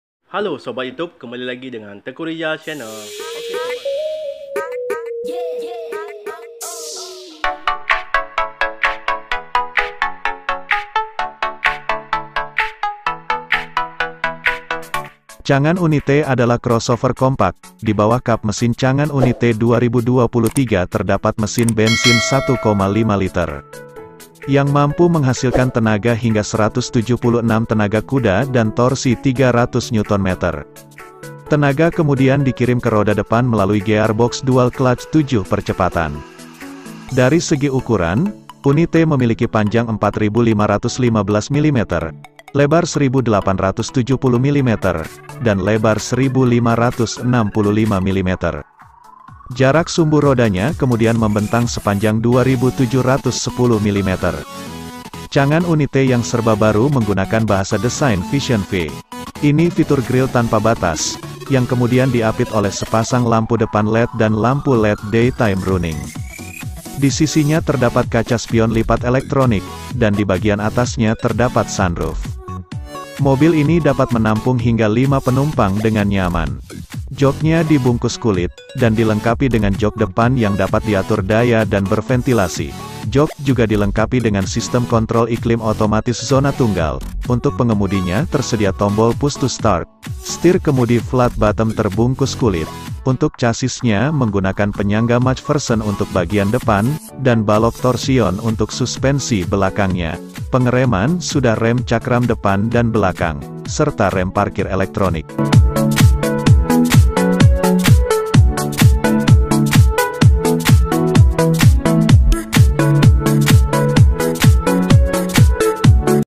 2023 Changan UNI - T, Mobil China Lebih Mewah dari All New HR-V, Harga 250 Juta, HR-V Panik
2023 Changan UNI - T,  Mobil China Lebih Mewah dari All New HR-V, Harga 250 Juta, HR-V Panik

Changan Uni-T adalah crossover kompak.

 Di bawah kap mesin Changan Uni-T 2023 terdapat mesin bensin 1,5 liter,

 yang mampu menghasilkan tenaga hingga 176 tenaga kuda dan torsi 300 Nm.

Tenaga kemudian dikirim ke roda depan melalui gearbox dual-clutch tujuh percepatan.

 Dari segi ukuran, Uni-T memiliki panjang 4.515mm, lebar 1.870mm, dan lebar 1.565mm.

Jarak sumbu rodanya kemudian membentang sepanjang 2.710 mm.

Changan Uni-T yang serba baru menggunakan bahasa desain Vision V.

 Ini fitur gril tanpa batas, yang kemudian diapit oleh sepasang lampu depan LED dan lampu LED daytime running.

Di sisinya terdapat kaca spion lipat elektronik, dan di bagian atasnya terdapat sunroof.

mobil ini dapat menampung hingga lima penumpang dengan nyaman.

 Joknya dibungkus kulit, dan dilengkapi dengan jok depan yang dapat diatur daya dan berventilasi.

jok juga dilengkapi dengan sistem kontrol iklim otomatis zona tunggal.

  untuk pengemudinya tersedia tombol push-to-start,

stir kemudi flat-bottom terbungkus kulit,

untuk chasisnya menggunakan
penyangga MacPherson untuk bagian depan,

dan balok Torsion untuk suspensi belakangnya.

Pengereman sudah rem cakram depan dan belakang, serta rem parkir elektronik.